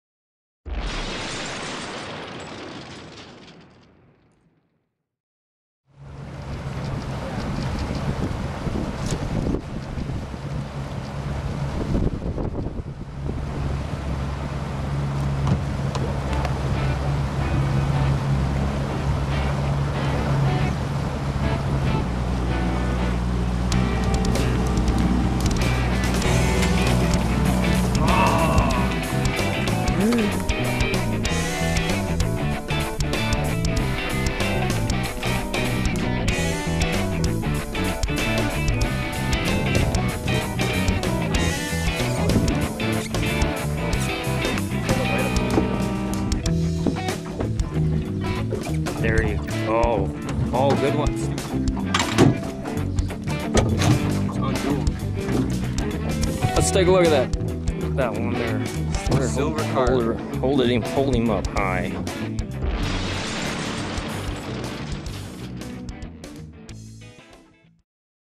Air born Asian Carp Shot while bowfishing Missouri River
Paul Korn A-1 Archery & Stick the fish PRESENTS: Stewart Korn Bow Fishing the Missouri River July 2013 with our good friend Marlyn Wiebelhaus of Weibelhaus Guiding in Wynott Nebraska .  In this video Stewart takes an Asian Carp or flying carp as they are sometimes called out of the air with his bow fishing set up using a retriever bowfishing reel.  We were not going after these fish on this trip, but when we got to a hole they were in Marlyn would run thru it and get a few jumping a real fun break from floating the banks from time to time...